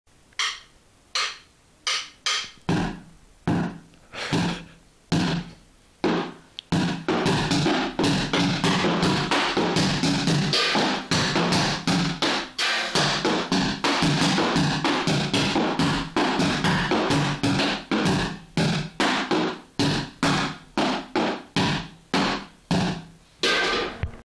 Schlagzeugsolo von Emanuel (8Jahre)
Er ist erst 8 Jahre, hat sich in seinem Zimmer ein Schlagzeug gebaut und zeigt hier was er drauf hat.. Ein Naturtalent :)